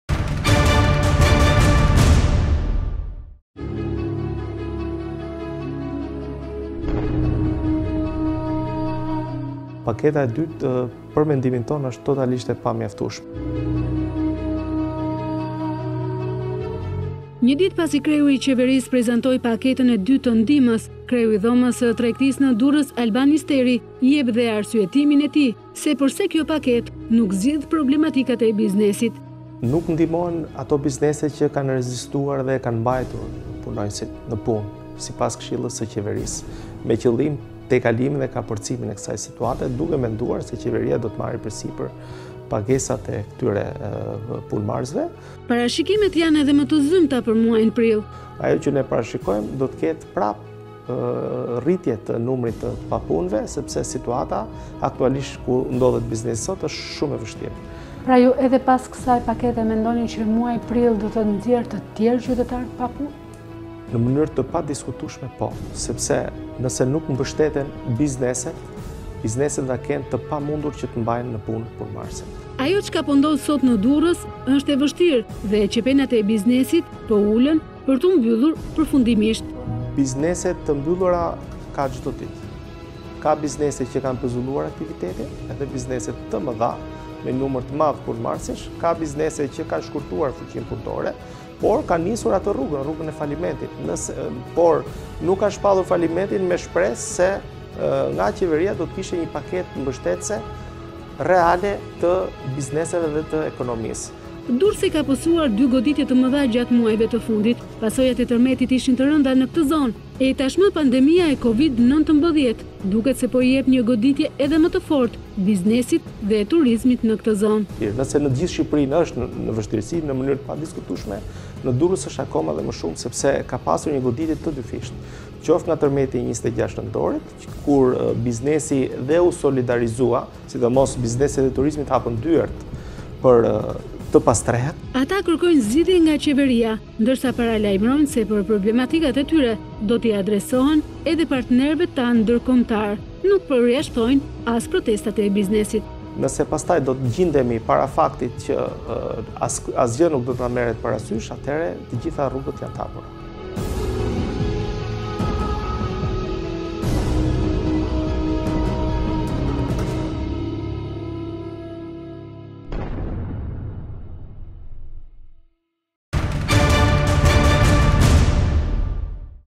Tempora - Biznesi i Durrësit: Paketa e Ramës nuk zgjidh problemet do ketë shumë të papunë
Një ditë pasi kreu i qeverisë prezantoi paketën e dytë të ndihmës, kreu i Dhomës së Tregtisë në Durrës, Alban Isteri jep dhe arsyetimin e tij se përse kjo paketë nuk zgjidh problematikat e biznesit.

“Paketa e dytë është totalisht e pamjaftueshe. Nuk ndihmon biznesin që kanë rezistuar dhe e kanë mbajtur biznesin në punë me qëllim tejkalimin e kësaj situate duke menduar që pagesat do të merrte përsipër qeveria” thotë ai.

Parashikimet janë edhe më të zymta për muajin prill.